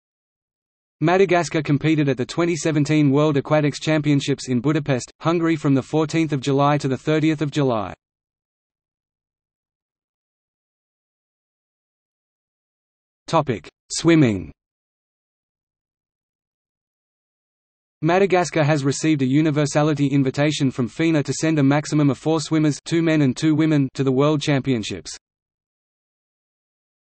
Madagascar at the 2017 World Aquatics Championships | Wikipedia audio article
00:00:09 Swimming

- Socrates

SUMMARY
Madagascar competed at the 2017 World Aquatics Championships in Budapest, Hungary from 14 July to 30 July.